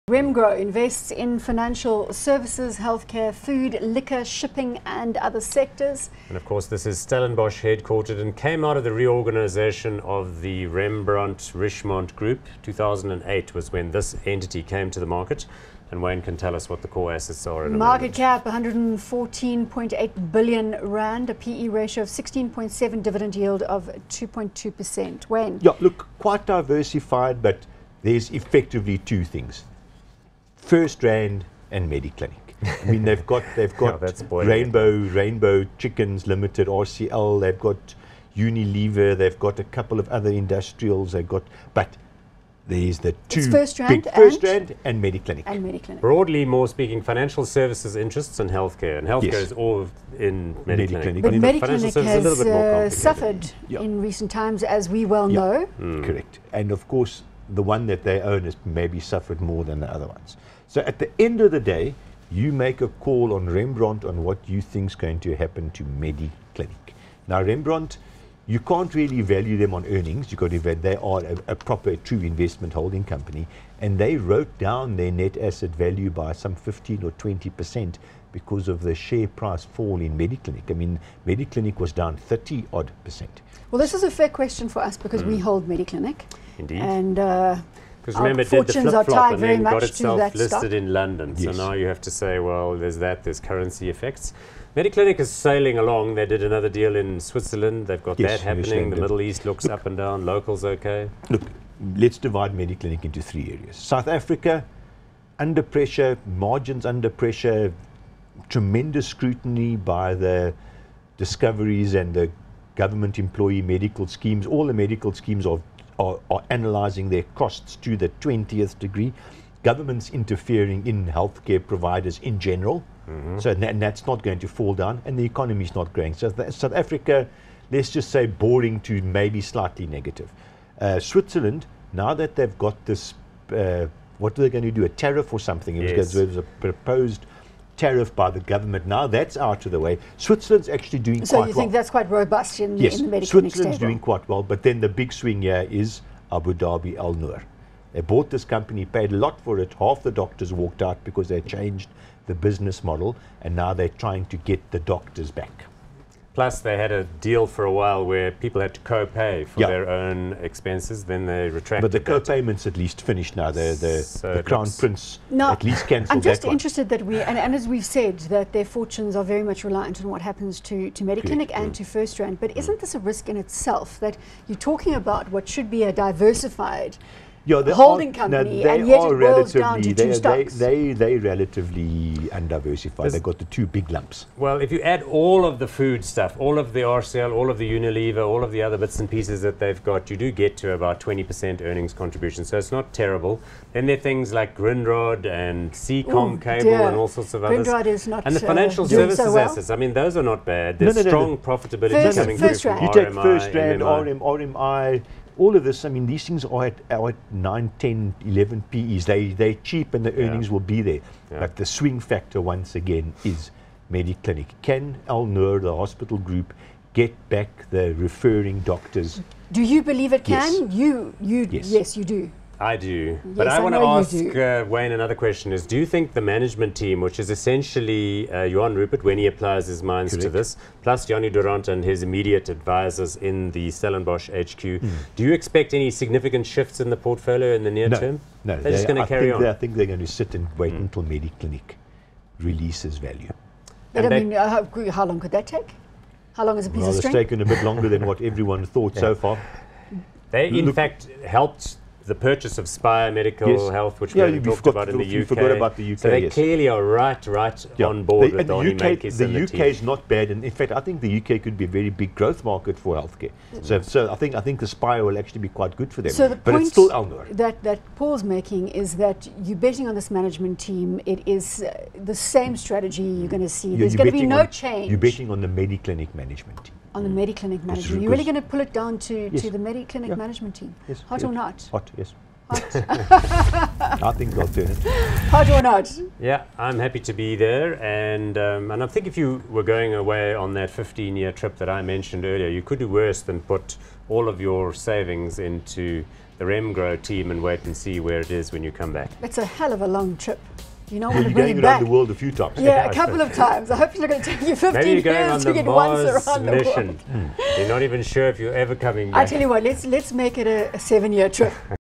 Remgro - Hot or Not
Remgro invests in financial services, healthcare, food, liquor, shipping and other sectors. It has a market cap of R114.8 billion, a PE ratio of 16.72 and a dividend yield of 2.16 per cent. Guest Wayne McCurrie, Senior Portfolio Manager at Ashburton Investments and resident expert Paul Theron from Vestact decide whether Remgro is hot or not.